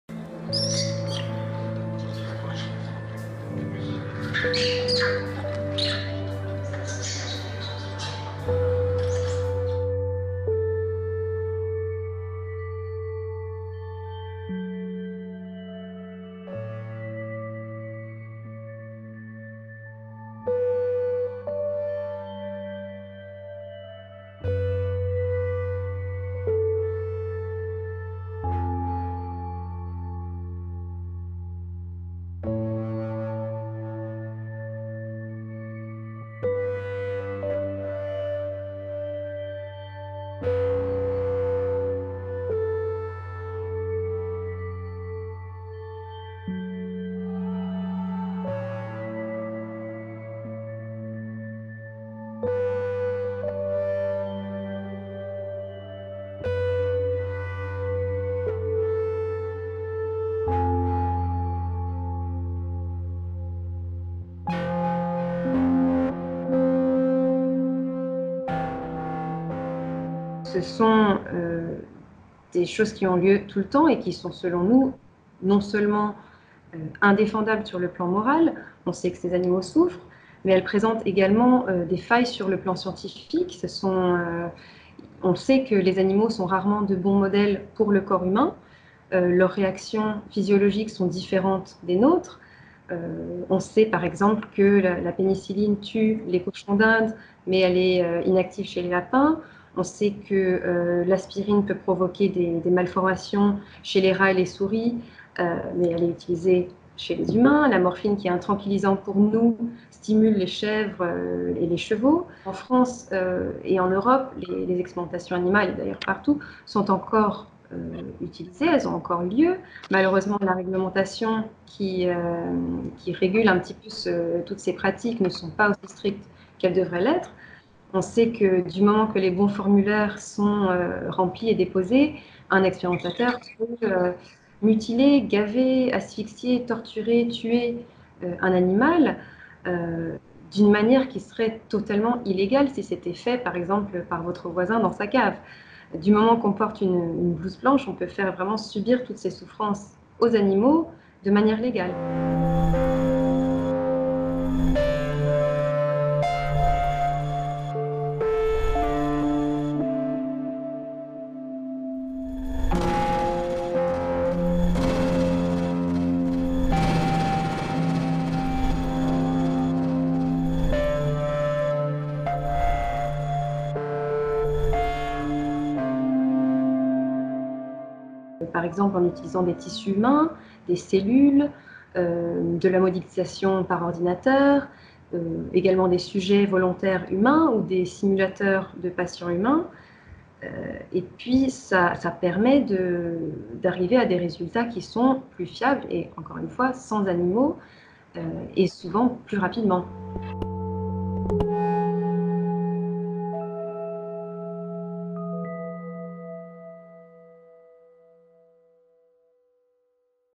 Expérimentation animale : une cruauté « qui a des failles sur le plan scientifique »
Chiens ensanglantés, singes assis dans des positions contraignantes et le cou dans un carcan, cages trop exiguës... Pendant plusieurs mois, un activiste sensible à la cause animale a travaillé dans un laboratoire de recherche près de Hambourg, dans le nord de l'Allemagne. Les images qu'il a tournées entre décembre 2018 et mars 2019, en caméra cachée, sont édifiantes. Malheureusement, en France et en Europe, les expérimentations animales sont toujours d'actualités. Pourtant, des alternatives existes, comme nous l'explique l'association PETA.

Vous avez une vidéo à nous proposer ? Vous souhaitez nous contacter ?